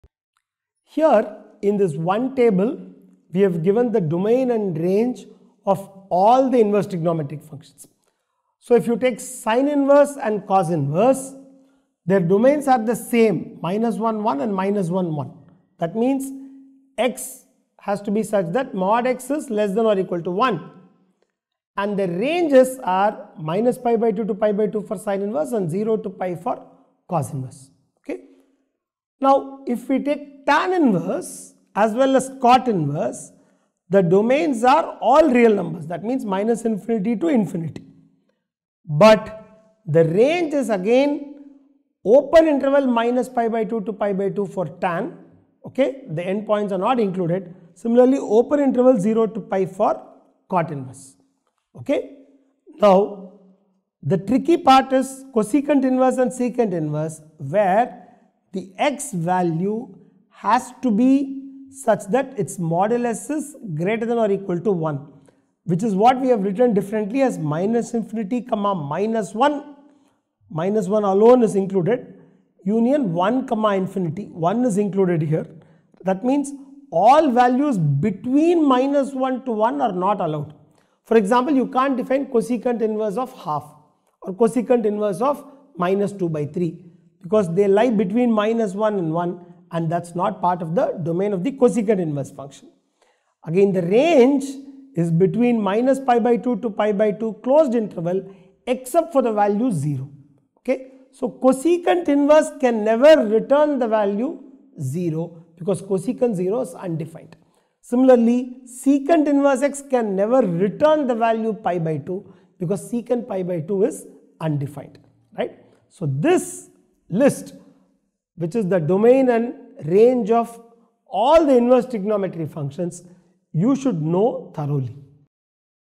Domain and Range of Trigonometric Functions Class 12 | Maths | Chapter 2 | AhaGuru Ravishankar
🧠 Confused about the domain and range of inverse trigonometric functions? We’ve got you covered!

In this video, we break down the domain and range of all six inverse trig functions using simple logic, visuals, and memory tricks. Whether you’re preparing for CBSE Boards or JEE, this video will make it super easy to remember and apply these rules.

💡 What You’ll Learn:

🔖What is domain and range in inverse trig?
🔖sin⁻¹(x), cos⁻¹(x), tan⁻¹(x) – domain & range
🔖cot⁻¹(x), sec⁻¹(x), cosec⁻¹(x) – domain & range